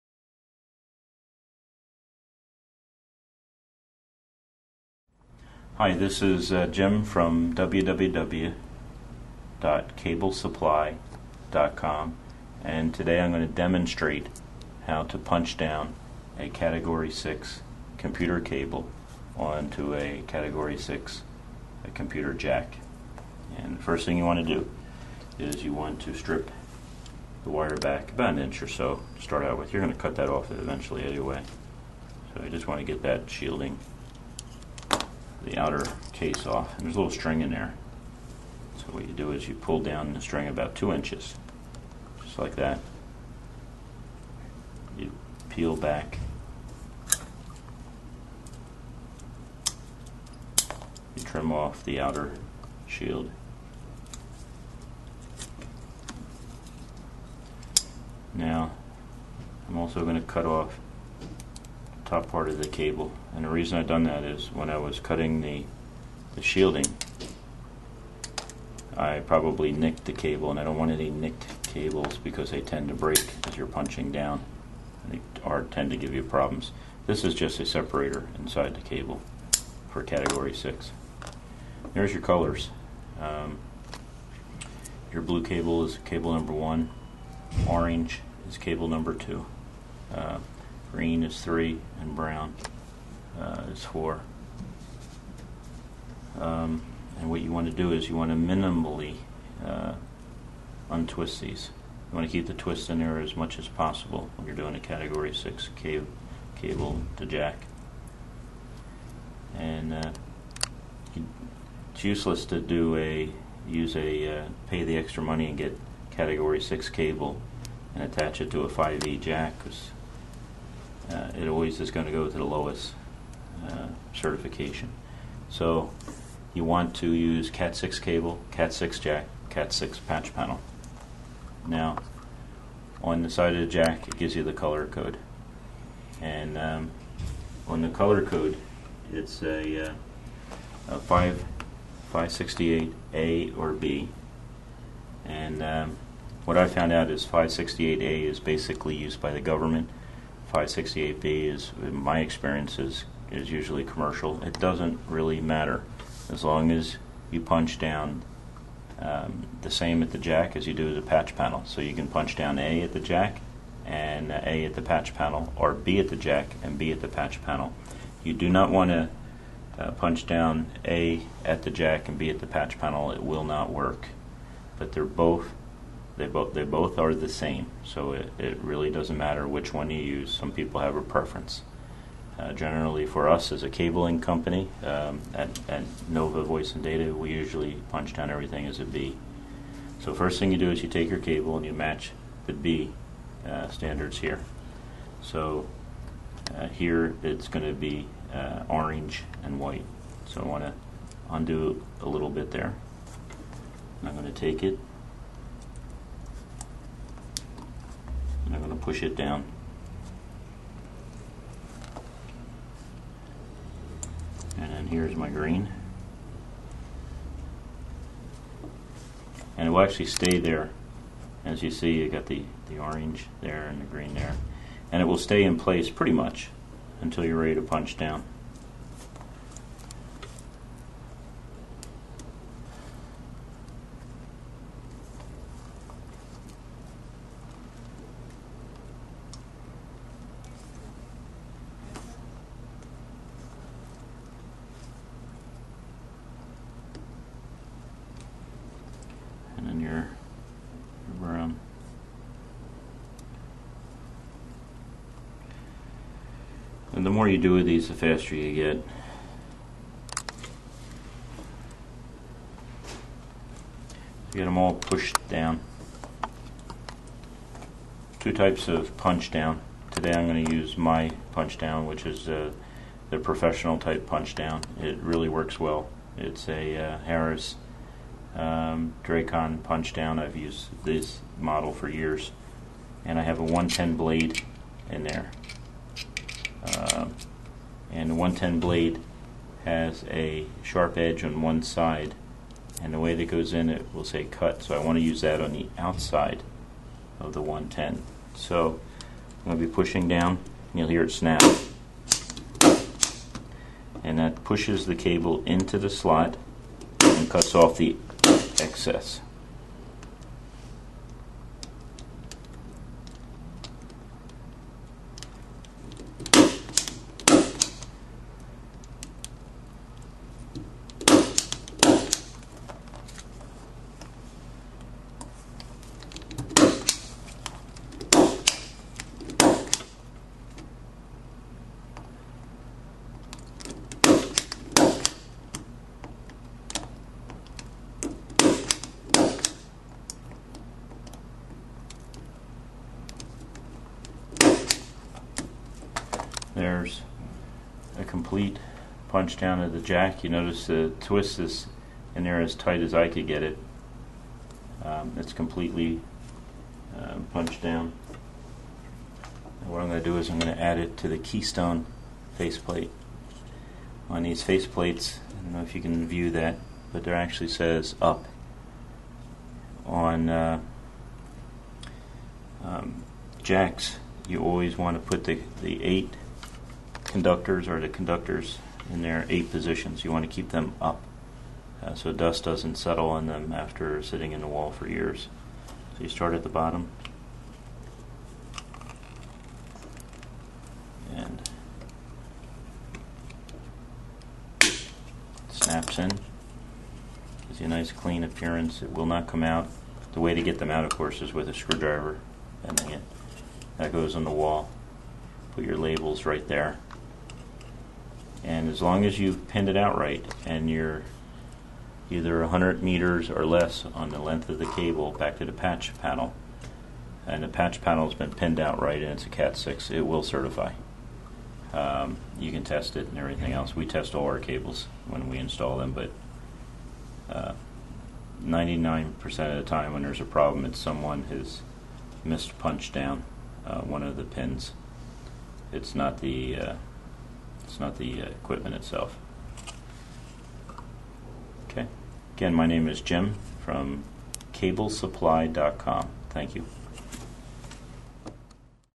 How to cable a Cat6 jack
In this video Jim Gibson will demonstrate how to punch down a Cat6 cable into a keystone jack with an impact tool. Item 100356

Cat6 jacks #104069
Cat5E Jacks #104068